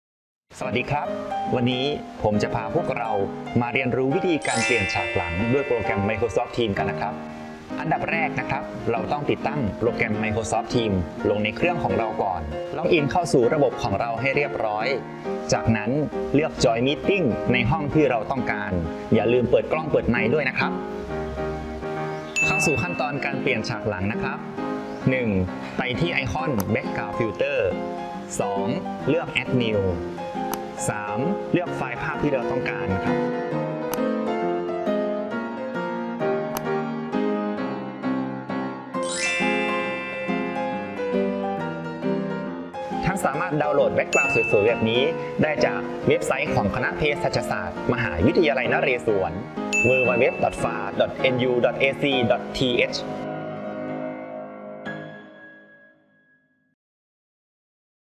เปลี่ยนฉากหลัง Microsoft Team ง่ายนิดเดียว !! (ep.1)
เพราะการเปลี่ยน ฉากหลัง Microsoft Team ง่ายนิดเดียว !!
พบกับ IT Guide by RxNU

พิเศษ!! สำหรับบุคลากร นักวิจัย และนิสิตคณะเภสัชศาสตร์
สามารถดาวน์โหลด " Background Screen "